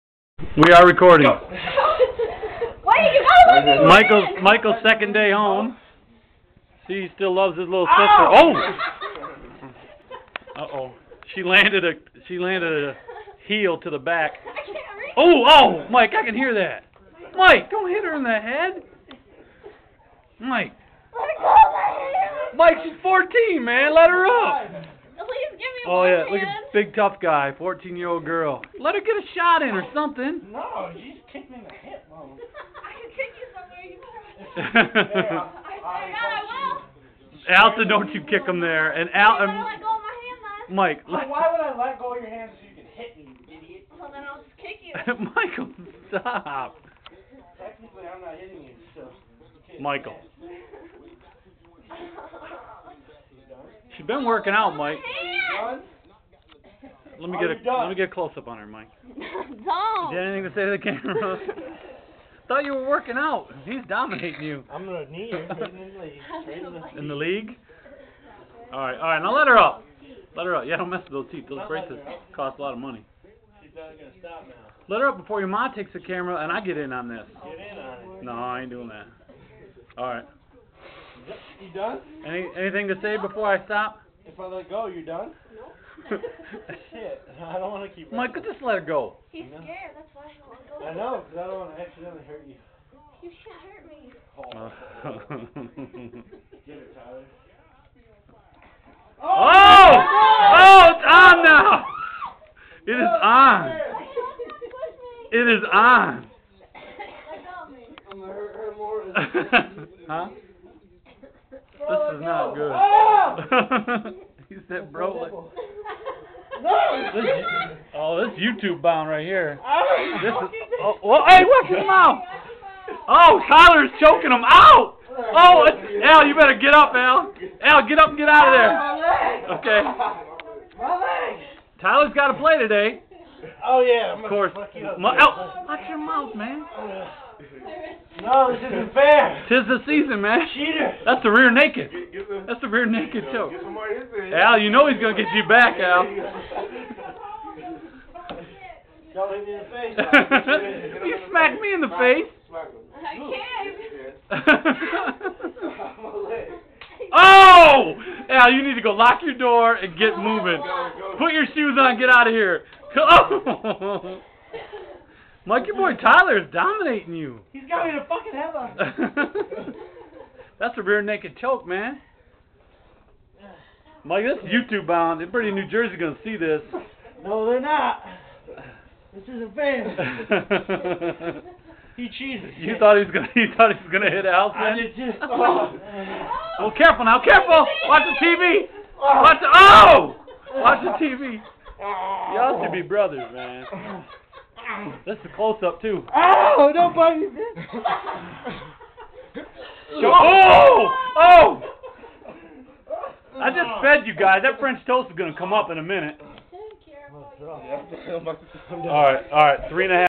Allison and Mike scrapping!!!
Mike has been home for a day and a half and decided to lay a beaten on his little sister I thought Tyler was gonna assist Michael in the fight but u will see I was wrong